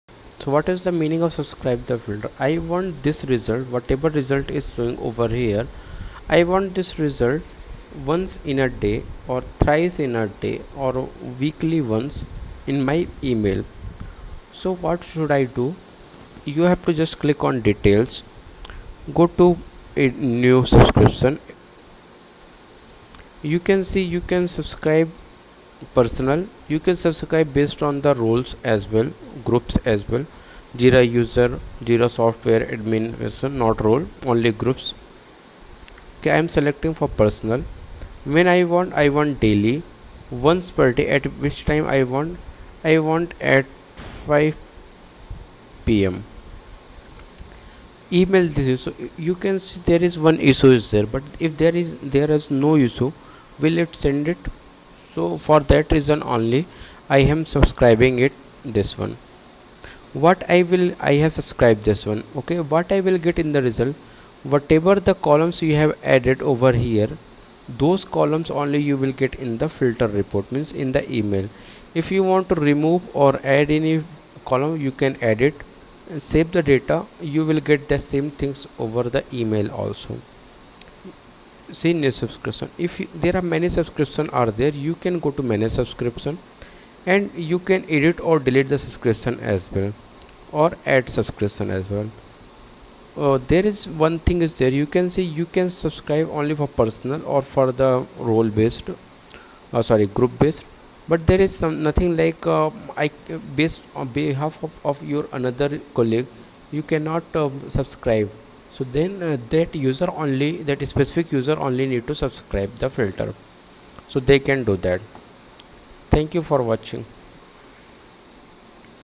Filter subscription in Jira. Get JQL result in email.
How to get the report by email?
How to get JQL to result in email?